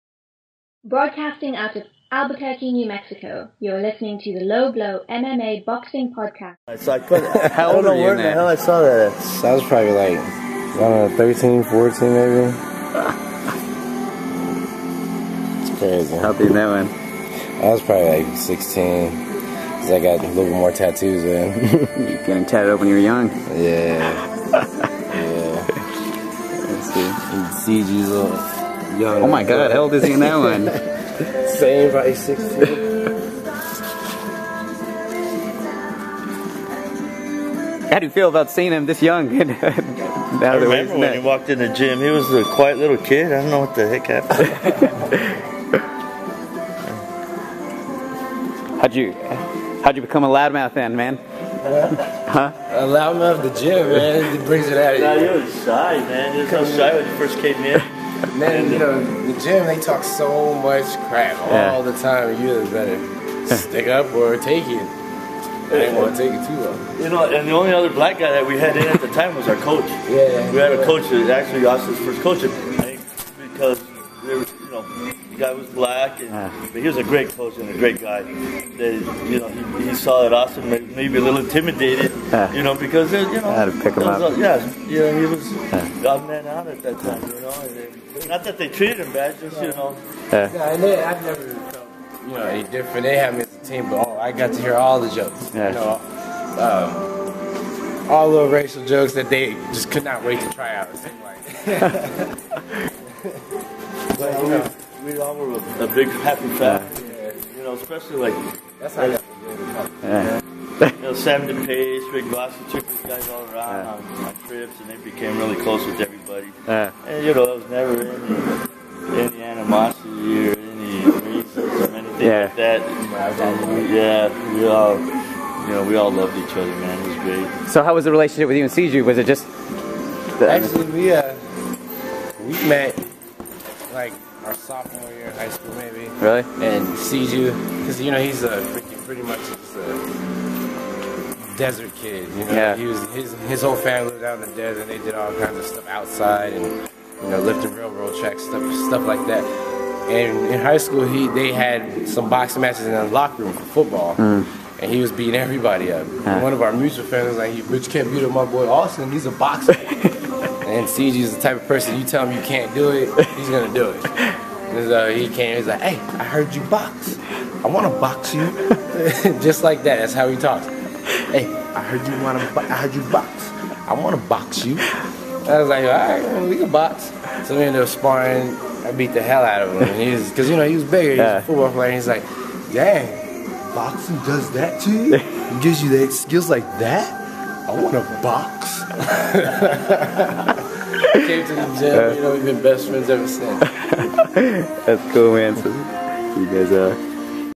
Austin Trout and Trainer Louie Burke Talk Trout's Start in Boxing
Austin Trout and his trainer Louie Burke on Trout and Siju Shabazz's start at the Las Cruces PAL gym. Trout will be facing Delvin Rodriguez in the second defense of his WBA Super Welterweight Championship in Carson, California June 2nd at the Home Depot Center.